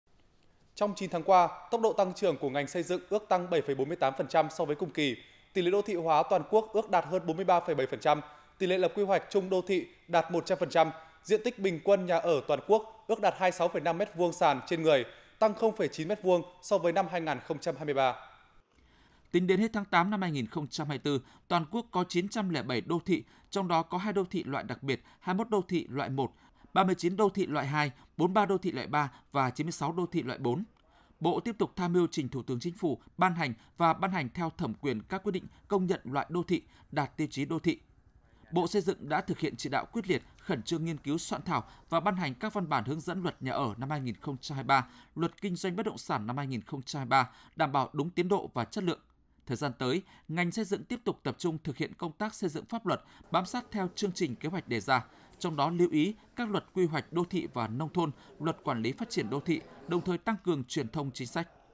Ngành xây dựng tăng trưởng 7,48% trong 9 tháng
Trong 9 tháng qua, tốc độ tăng trưởng của ngành Xây dựng ước tăng 7,48% so cùng kỳ. Tỷ lệ đô thị hóa toàn quốc ước đạt hơn 43,7%. Tỷ lệ lập quy hoạch chung đô thị đạt 100%. Diện tích bình quân nhà ở toàn quốc ước đạt 26,5m2 sàn/người, tăng 0,9m2 so với năm 2023.